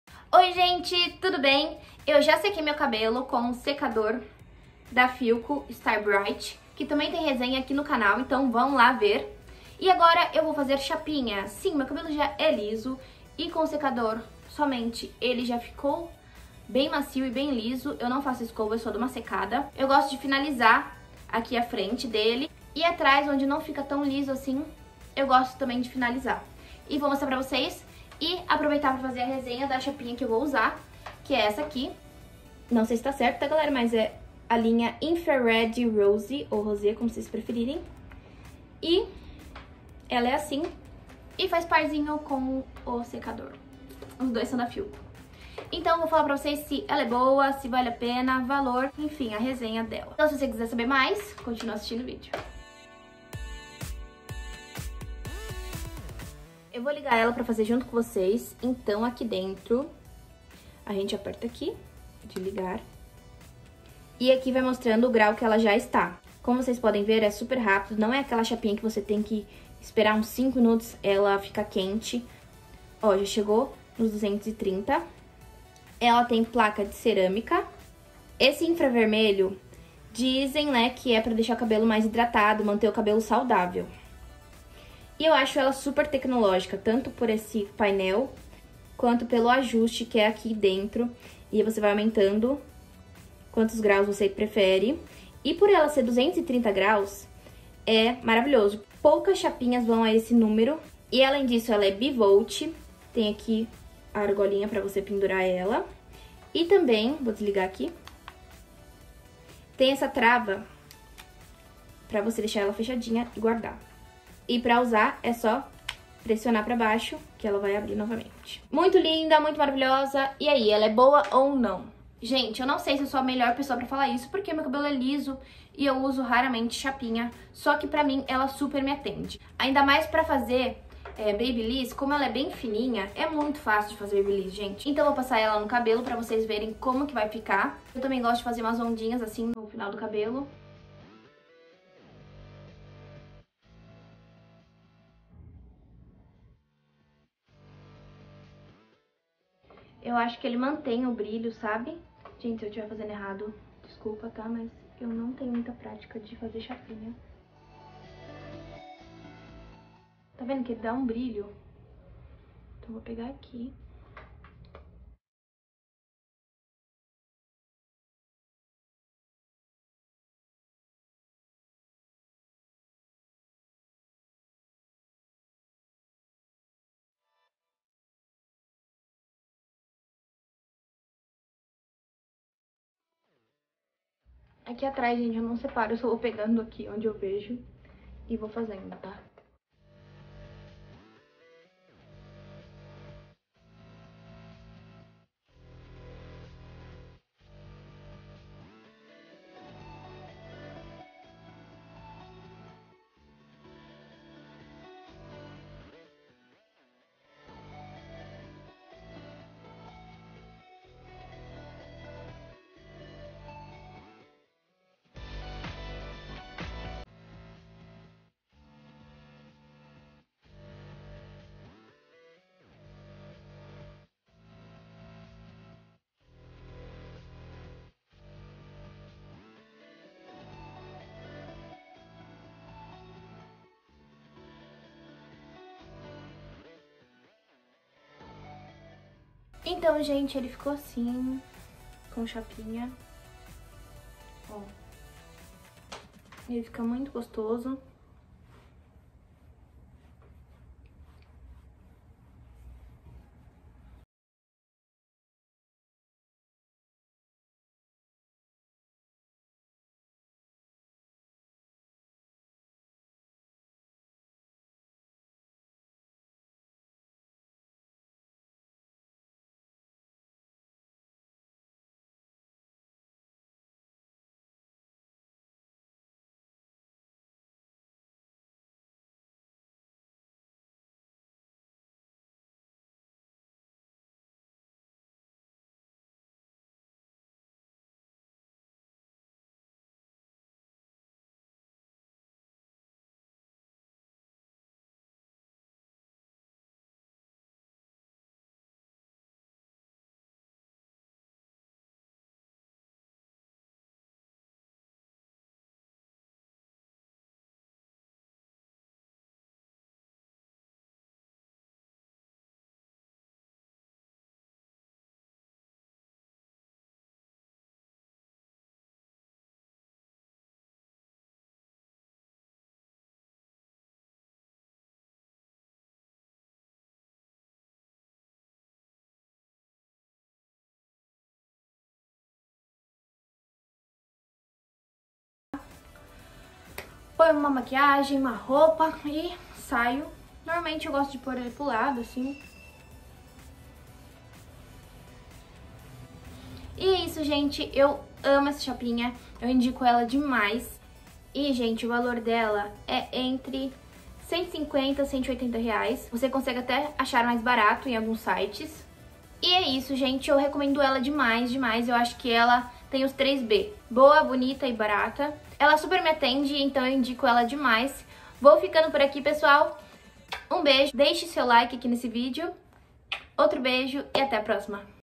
Resenha Chapinha InfraRed Rose da Philco | Qual Chapinha é Melhor para Comprar?
*música retirada no meio do vídeo por direitos autorais*